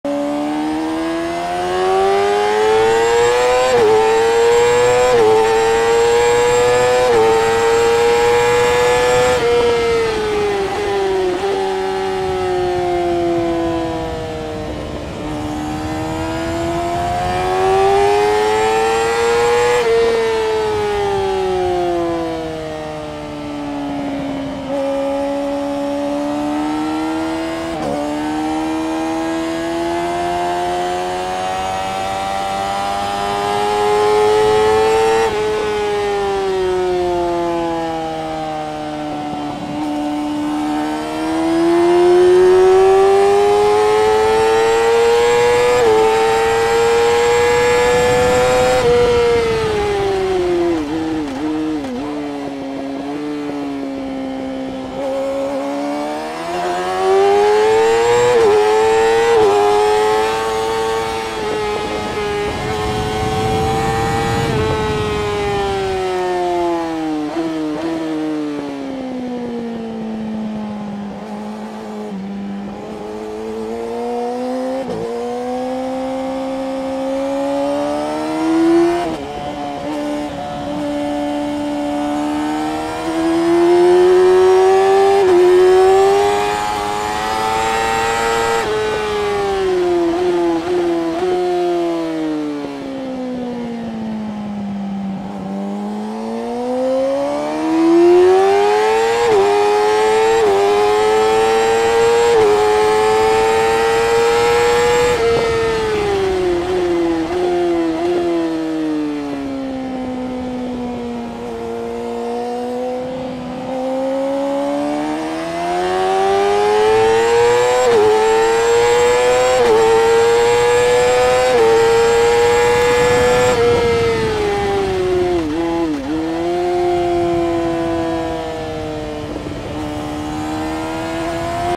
R6 1:54.3 - Onboard at Zolder with Motorsportschool Zolder - Session 4 Group 2 [2022.05.09]
R6 1:54.3 - Onboard at Zolder with Motorsportschool Zolder - Session 4 Group 2 [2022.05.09] Helmet Cam